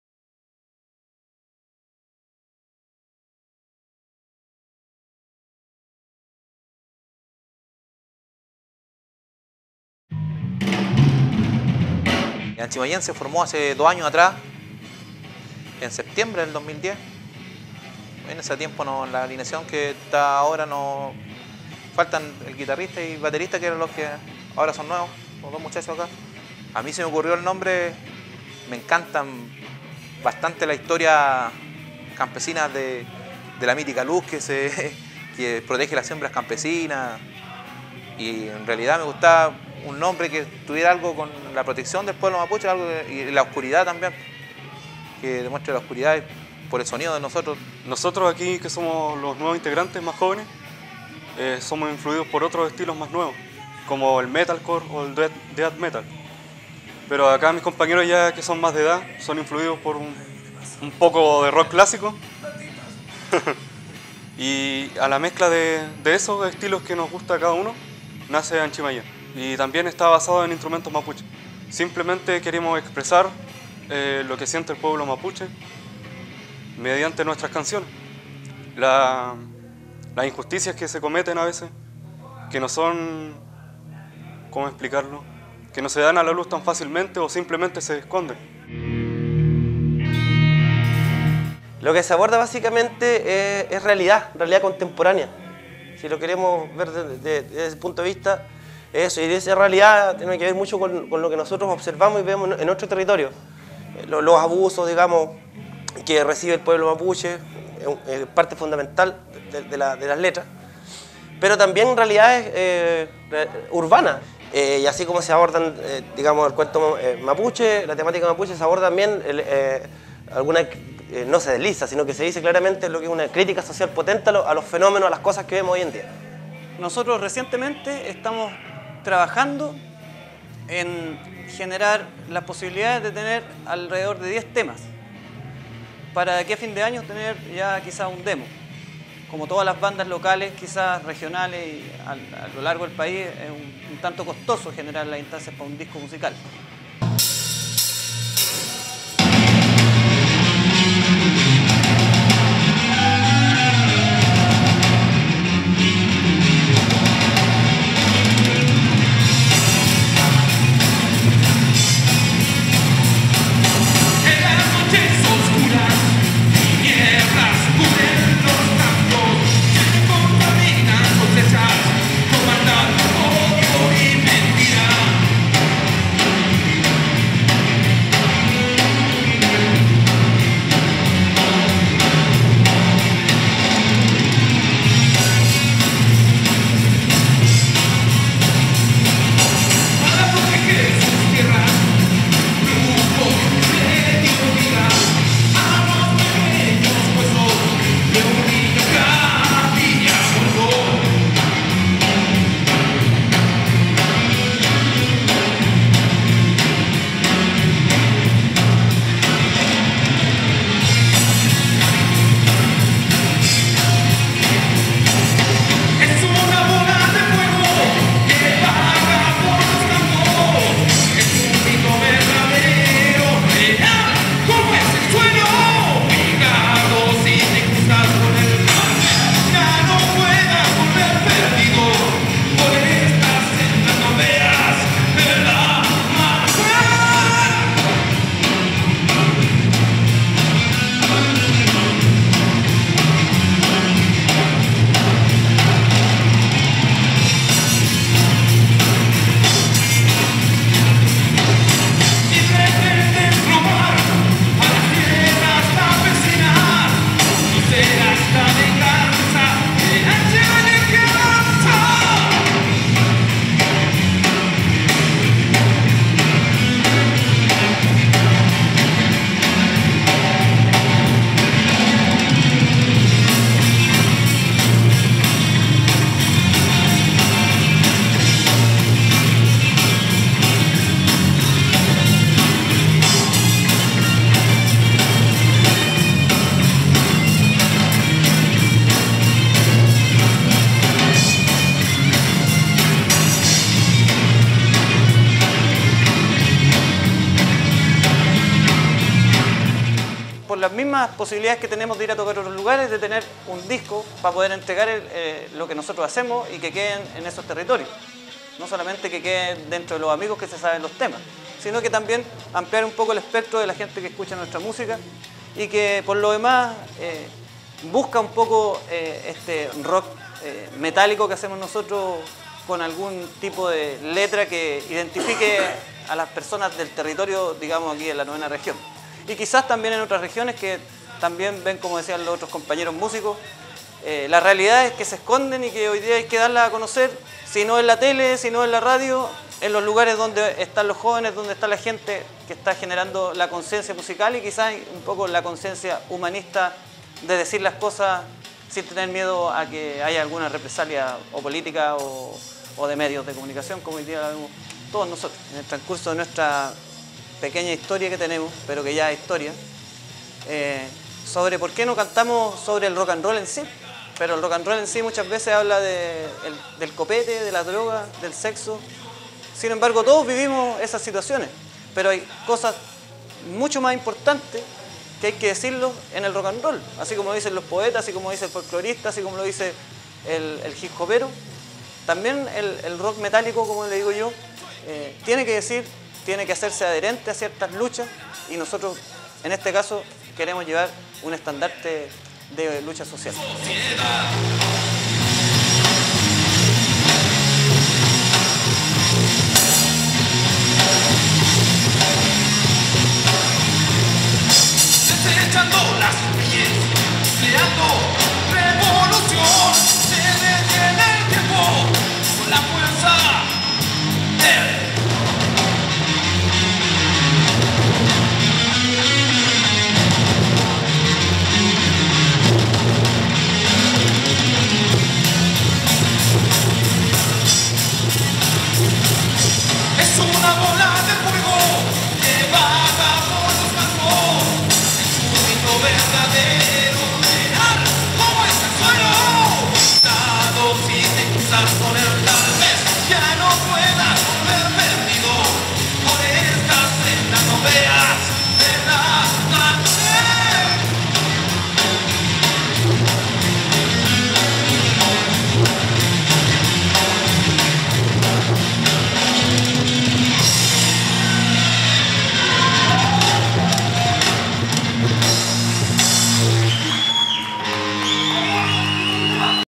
14 LEFTRARU MÚSICOS DE LA LLUVIA ANCHIMALLEN
Documental realizado el año 2012 en el desarrollo del taller audiovisual realizado en el liceo Jorge Teillier, de la ciudad de Lautaro, región de la Araucanía, Chile.
Dirección general: Carileo-Nova.